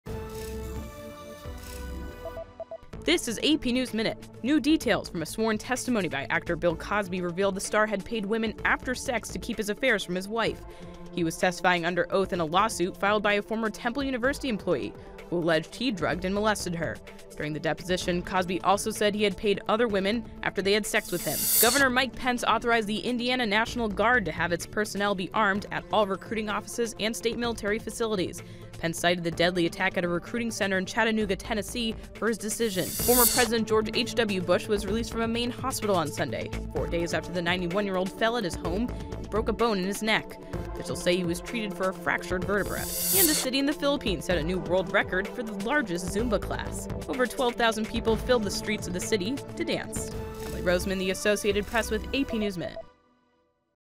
AP Top Stories July 19 p
Here's the latest for Saturday, July 19th: Cosby deposition reveals actor paid women to keep quiet; Indiana Gov. to arm National Guard; Former President George H.W Bush home from hospital; Record-breaking Zumba dance.